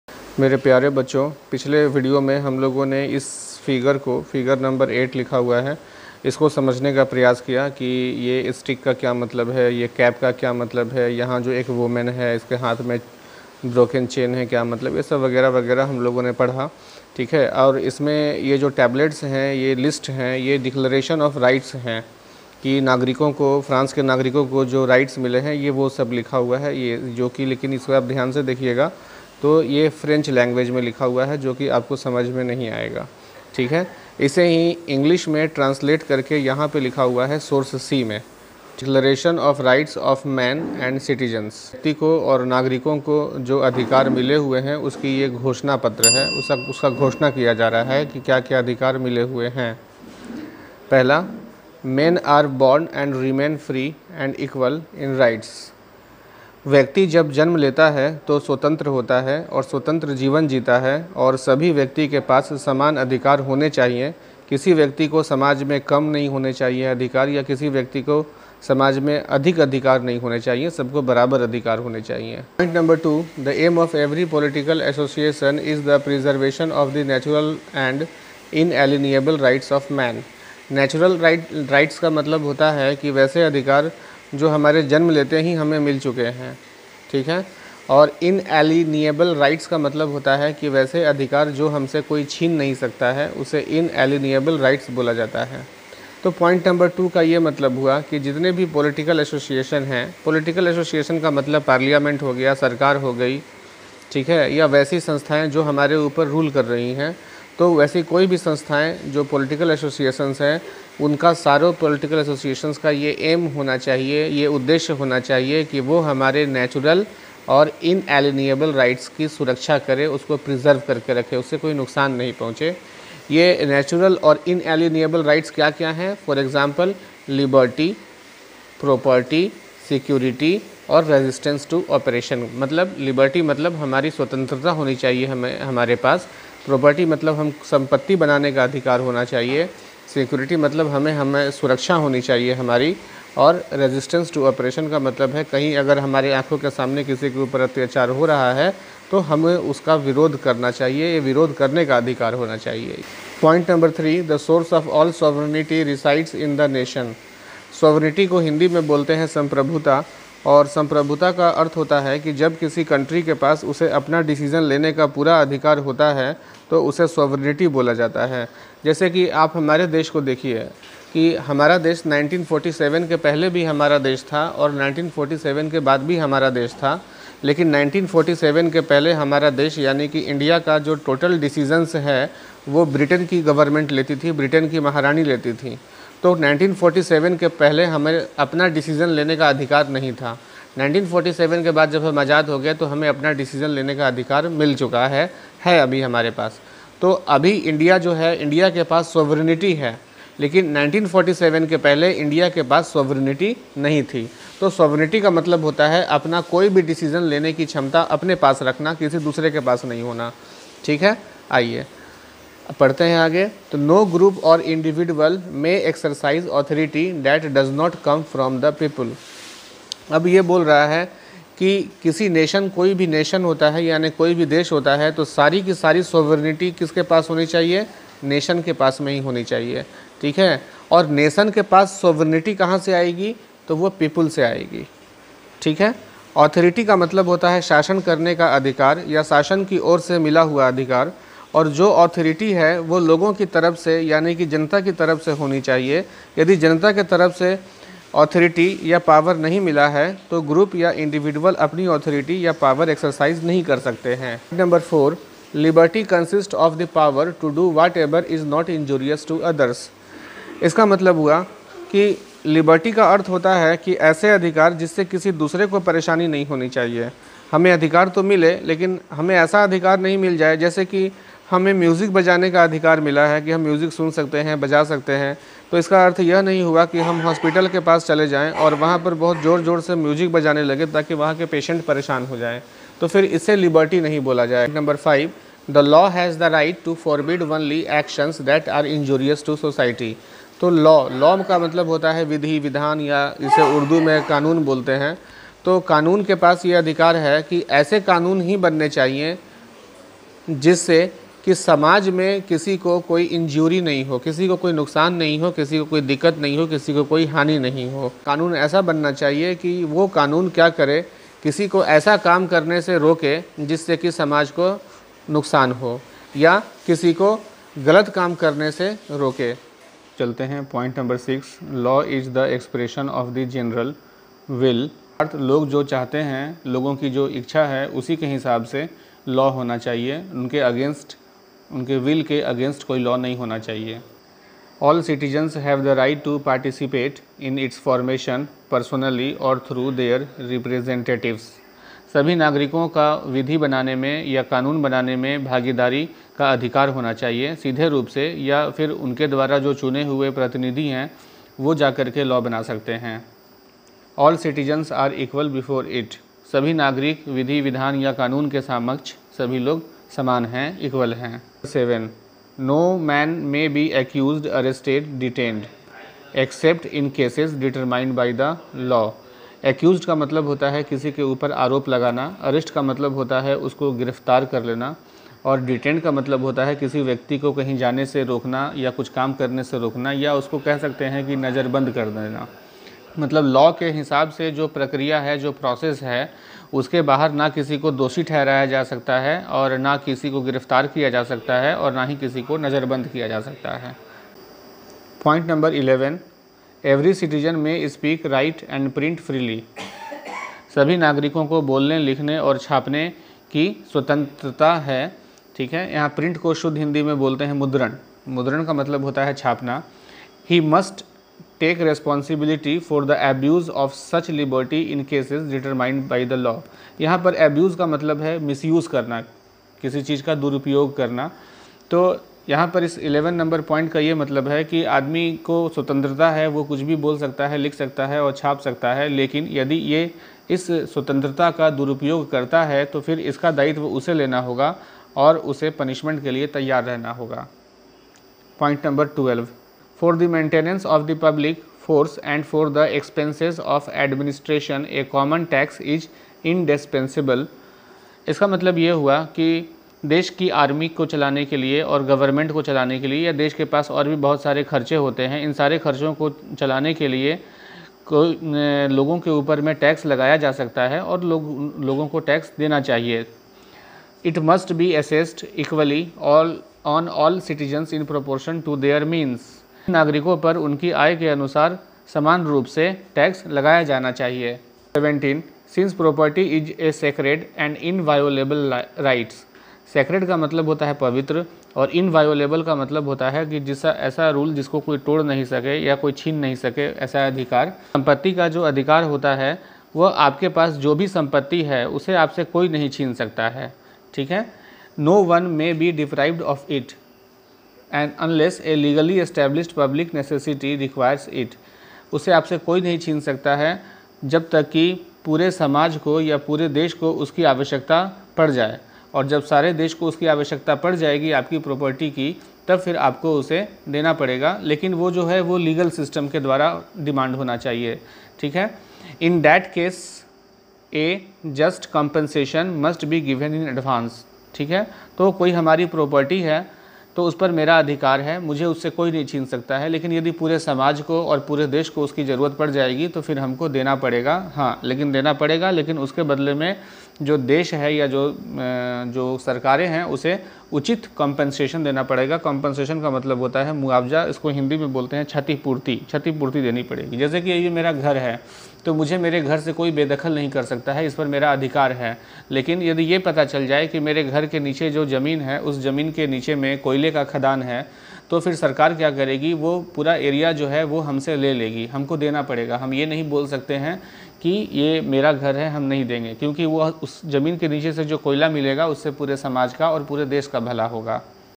The Declaration of Rights of Man and Citizen,part 7, chapter 1, history,class 9th,Aashish Vidyalay
this video explains the important rights of citizen painted by the artist Le Barbier in 1790 with reference to page 11, history ,CLASS- IX